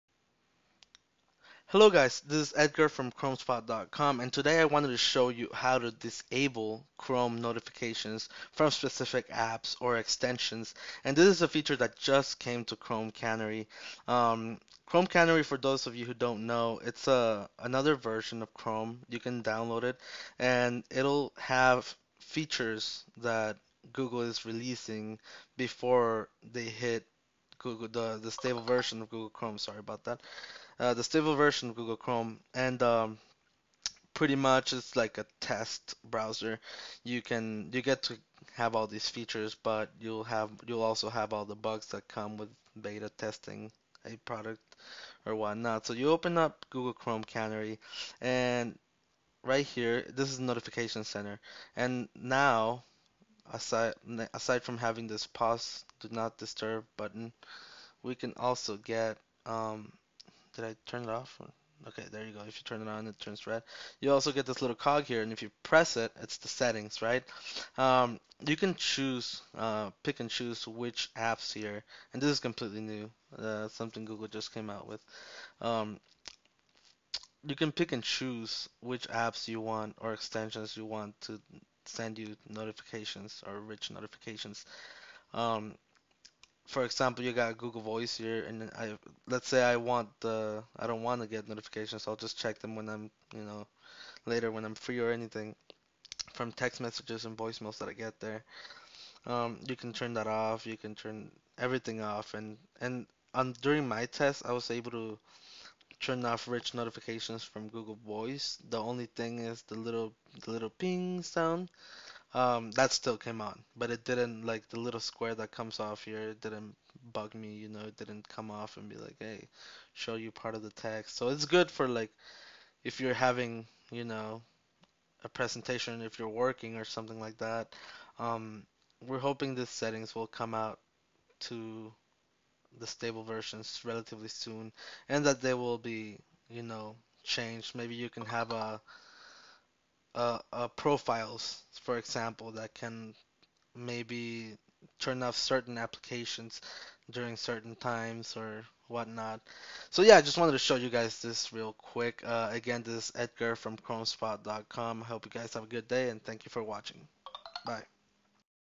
How to turn off Chrome notifications
Ever wanted to pick and choose which apps and extensions Chrome sends you notifications from? Now you can! The option is only available for Chrome Canary users, but it should be coming to everyone soon enough.